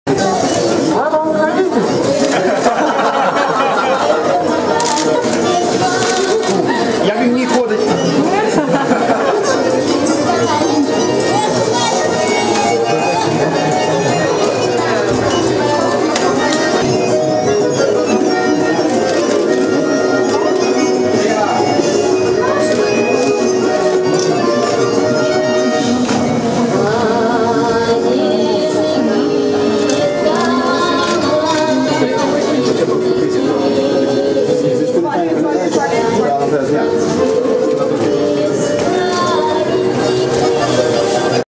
Убогий Радик в ролі Путіна
Львівські активісти Бойкоту російських товарів та тіпа-активіст Олег Радик у магазині ЦентрОбувь.